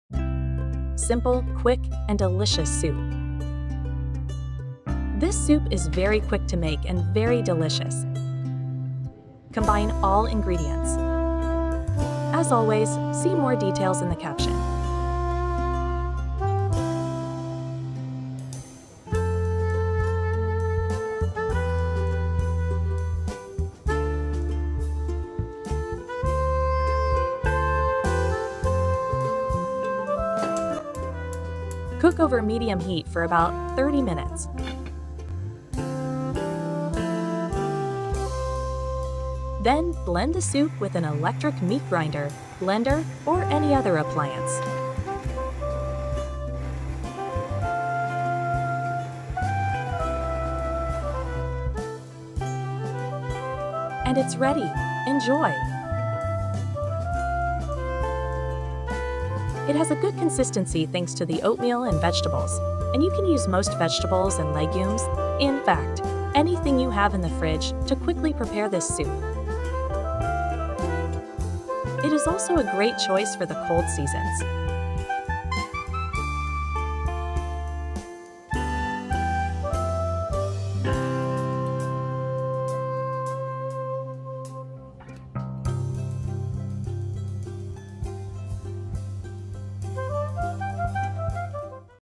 سوپ ساده و فوری | Simple, Quick, and Delicious Soup
This soup is very quick to make and very delicious. It has a good consistency thanks to the oatmeal and vegetables, and you can use most vegetables and legumes (in fact, anything you have in the fridge) to quickly prepare this soup. It is also a great choice for the cold seasons.

Ingredients:
• 2 medium carrots
• 2 small potatoes
• 1 onion
• 50 grams of peas
• 50 grams of rolled oats
• 1 teaspoon of turmeric
• 1/2 teaspoon of black pepper
• 2 cups of water

Instructions:
1. Combine all ingredients.
2. Cook over medium heat for about 30 minutes.
3. Then, blend the soup with an electric meat grinder, blender, or any other appliance.
4. Enjoy!

Tips:
• You can also use rolled oats.
• If you use paprika or bell pepper, you can change the color of your soup.

سوپ ساده، فوری و خوشمزه
• این مدل سوپ که بسیار سریع آماده میشه و خیلی هم خوشمزه‌س و به خاطر جو دوسر پرک و سبزیجاتش لعاب و غلظت خیلی خوبی هم داره و در عین حال شما می‌تونید با اکثر سیزیجات و حبوبات (یعنی در واقع هر چی که تو یخچال دارید) به سرعت این سوپ رو آماده کنید.
• ضمن اینکه برای فصل‌های سرد سال یه انتخاب عالیه.

مواد اولیه:
• هویج متوسط: ۲ عدد
• سیب‌زمینی کوچک: ۲ عدد
• پیاز: ۱ عدد
• نخودفرنگی: 50 گرم
• جو دوسر پرک: 50 گرم
• زردچوبه: 1 ق‌چ
• پودر فلفل سیاه:ن صف ق‌چ
• آب: ۲ لیوان

دستورالعمل:
• همه مواد رو ترکیب کنید.
• روی شعله متوسط حدود ۳۰ دقیقه قرار بدین.
• بعد با گوشت‌کوب برقی، مخلوط‌کن یا هر وسیله دیگه‌ای که میشه سوپ رو میکس کنید.
• و نوش جان.

ترفندها:
• می‌تونید از جو پرک هم استفاده کنید.
• اگه از پاپریکا یا فلفل دلمه‌ای استفاده کنید‌می‌تونید رنگ سوپ‌تون رو هم تغییر بدین.

Key Moments:
00:00 Intro
00:06 Add Vegetables
00:19 Add spices
00:31 Start cooking
00:37 Mix
01:02 Decoration and Serving